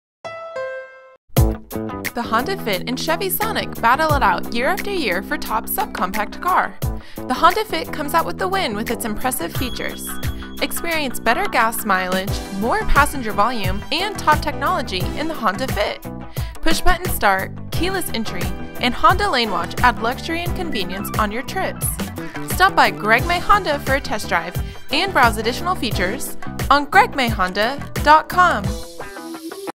Chevrolet Sonic vs. 2017 Honda Fit in Waco
The Honda Fit and Chevrolet Sonic battle it out year after year for top subcompact car. Today both these cars pack the best technology and features but one outdoes the other. Today Greg May Honda shows you how the 2017 Honda Fit in Waco leaves the Chevrolet Sonic in the dust with its impressive features.

Greg May Honda
1601 W Loop 340